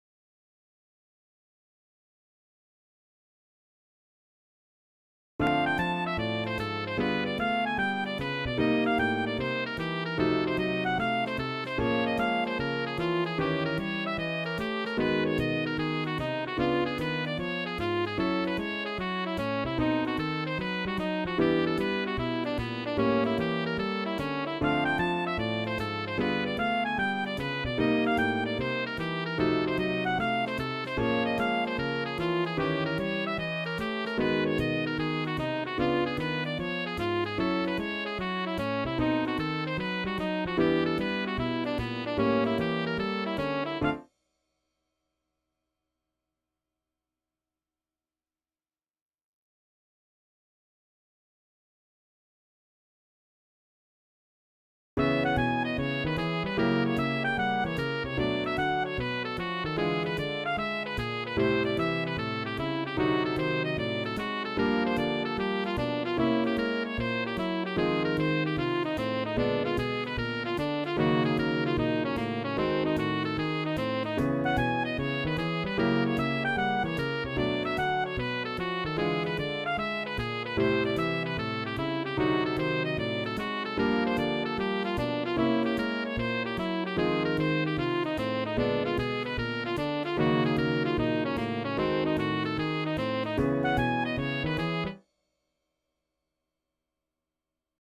ii-V around the cycle (2 bars per ii-V)
ii-V progression around the cycle using 2 bars per ii-V. Each ii-V advances two steps around the cycle, which is why we need two separate orbits.